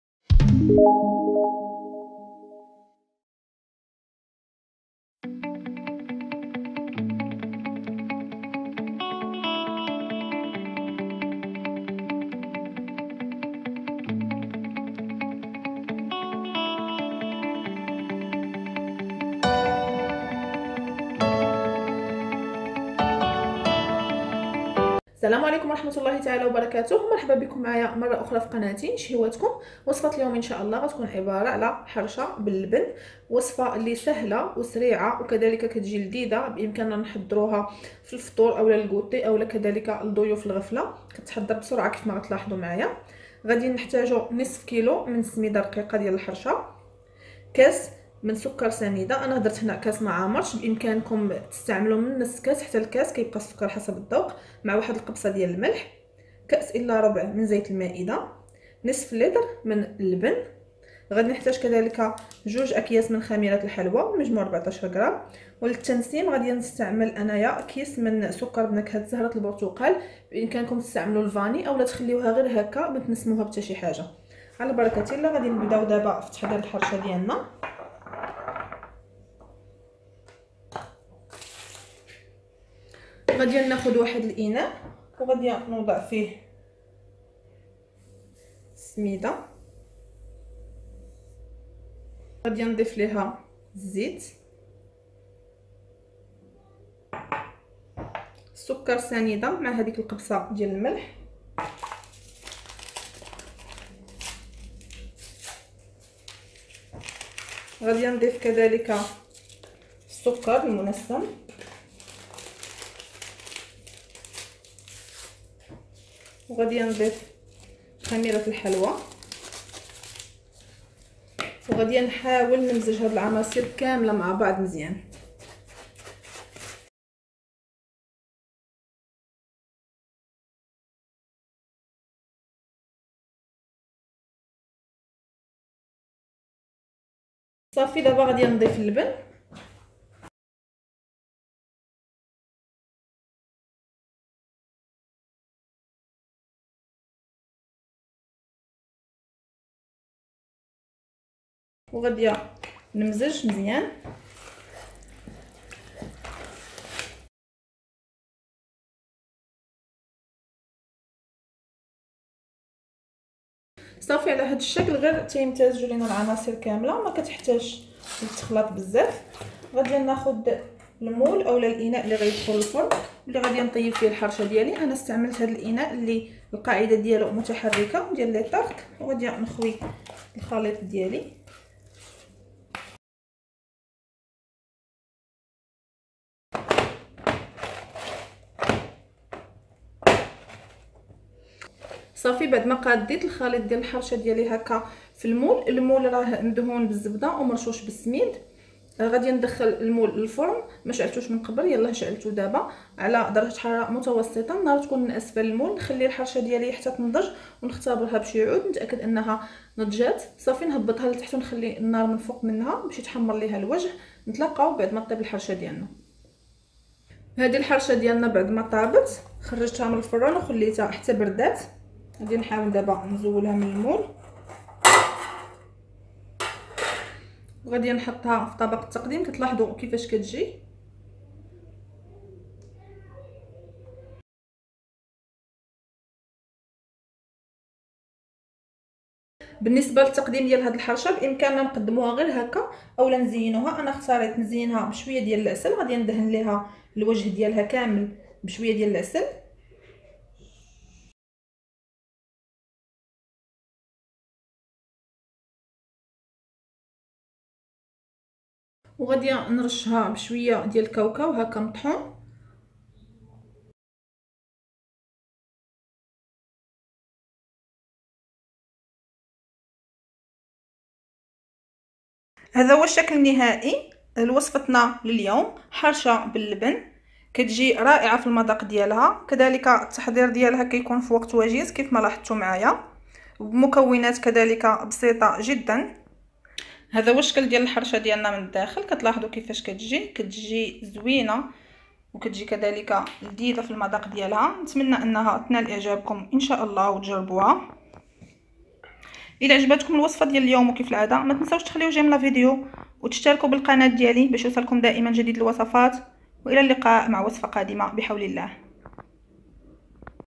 حرشة باللبن في دقائق لضيوف الغفلة هشيشة و مذاق راااائع
شهيواتكم تقدم لكم  حرشة باللبن في دقائق لضيوف الغفلة هشيشة و مذاق راااائع
لاتنسوا الإشتراك في القناة ليصلكم الجديد